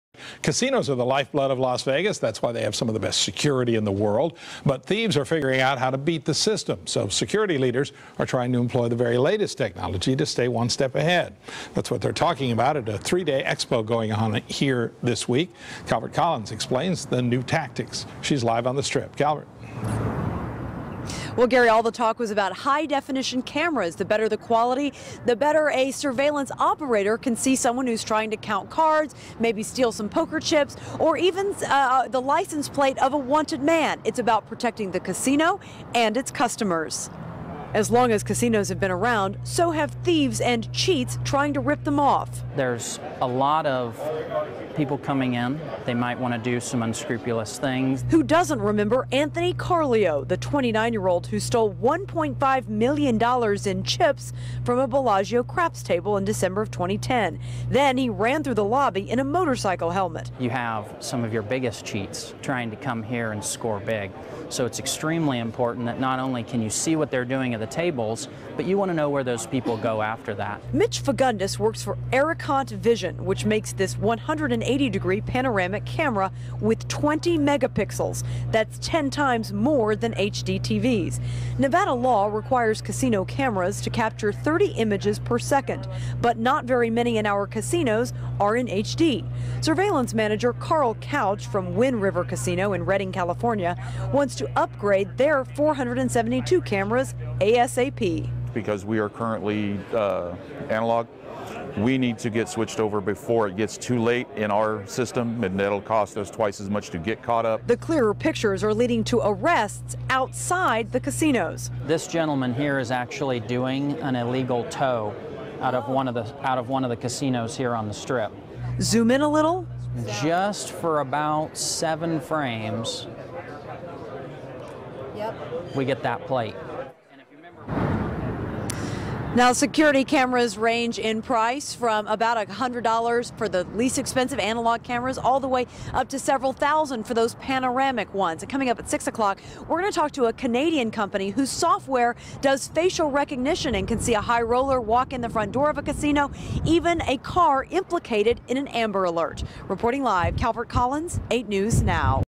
EVE Media Player in the News - Forensic Video Demonstration
The EVE Media Player from Network Optix being used to demonstrate HD Surveillance applications at the World Game Protection Conference in Las Vegas.  One of the pre-eminent shows for the casino/gaming security and surveillance industry.